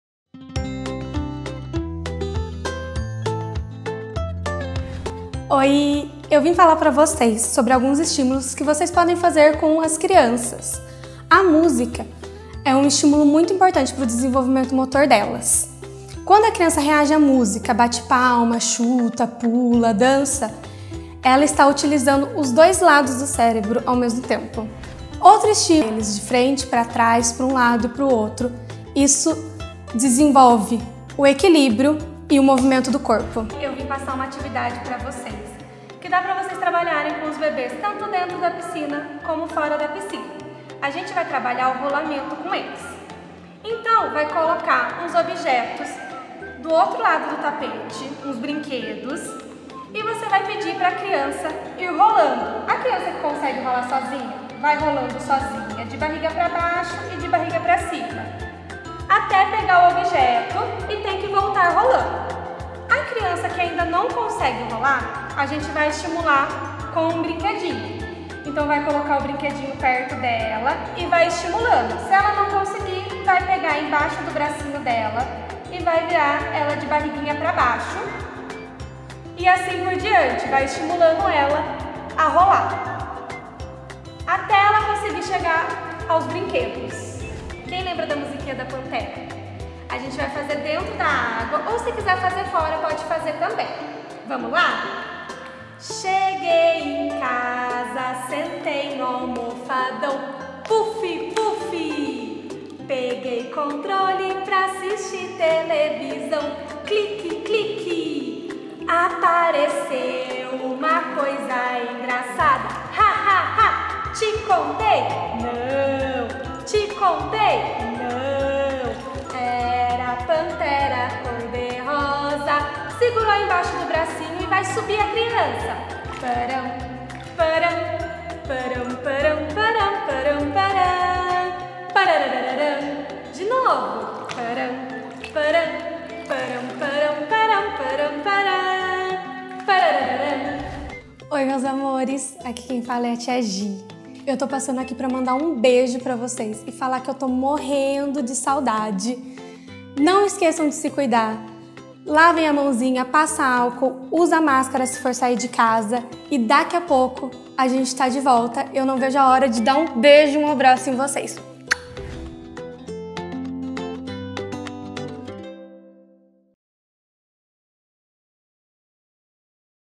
Natação Infantil - Exercícios para bebês
Nossa professora de natação infantil Giovanna Soares, deixou aqui alguns exercícios para estimular o desenvolvimento motor e de equilíbrios dos nosso pequenos.
Ela também deixou uma mensagem especial para seus alunos.

Confira, curta e compartilhe com a família e amigos!